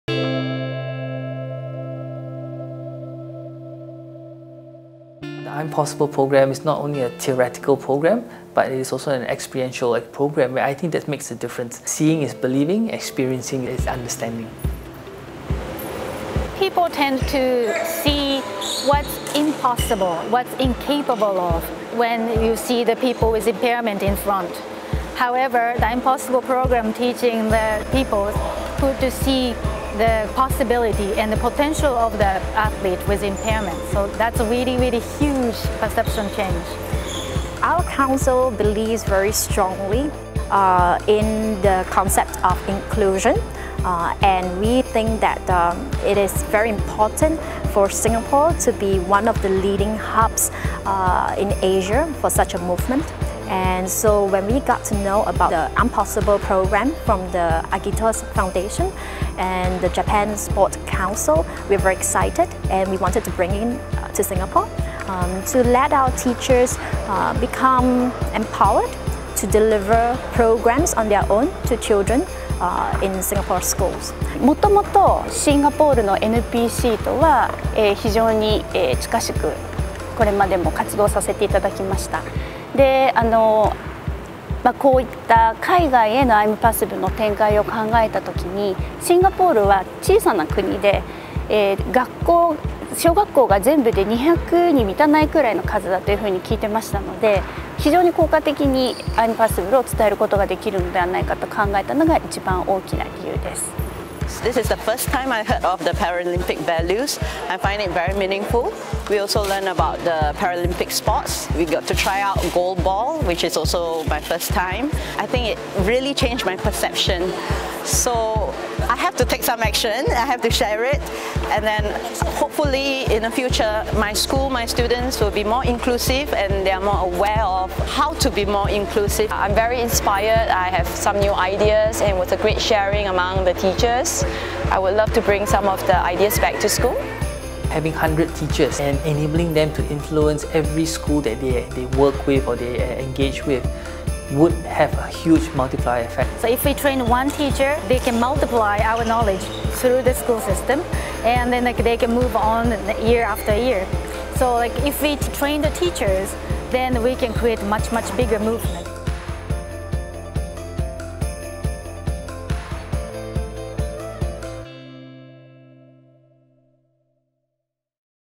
Promoting the Paralympic Values in Singapore | I'mPOSSIBLE | IPC's Educational Programme
Singapore is one of the countries implementing I’mPOSSIBLE, the education programme developed by the International Paralympic Committee to spread the Paralympic values and the vision of the Paralympic Movement to young people throughout the world. In November the Agitos Foundation hosted a teacher training to prepare them for the implementation of the programme.